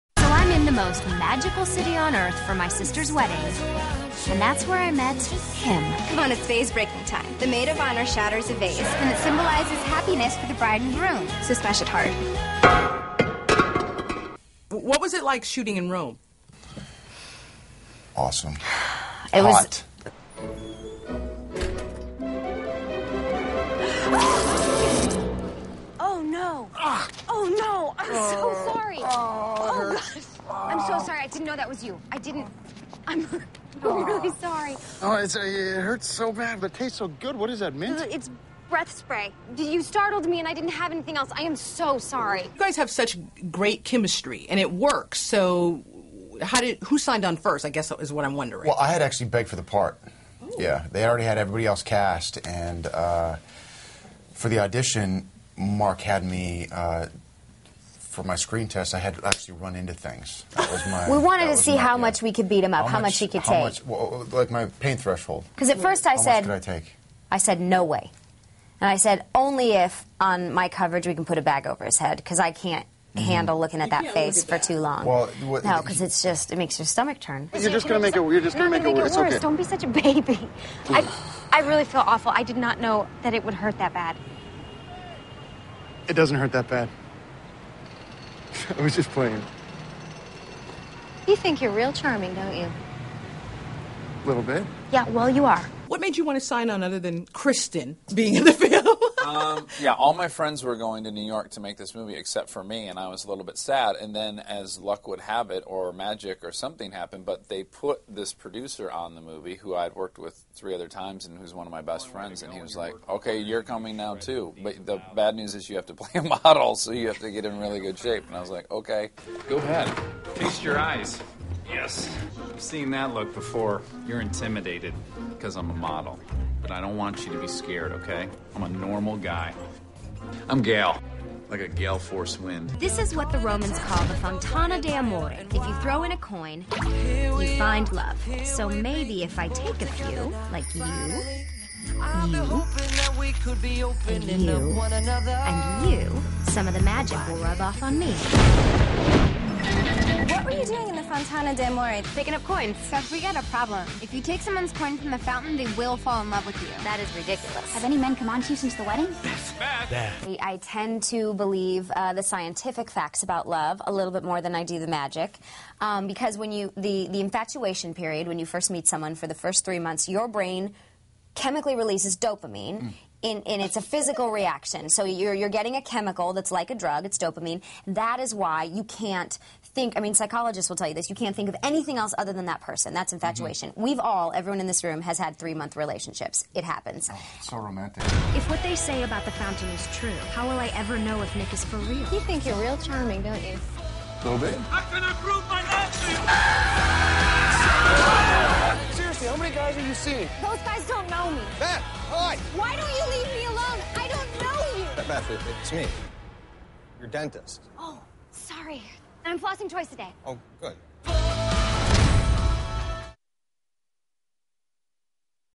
'When in Rome' cast speaks
Entertainment Producer KJ Matthews chats with the cast of "When in Rome."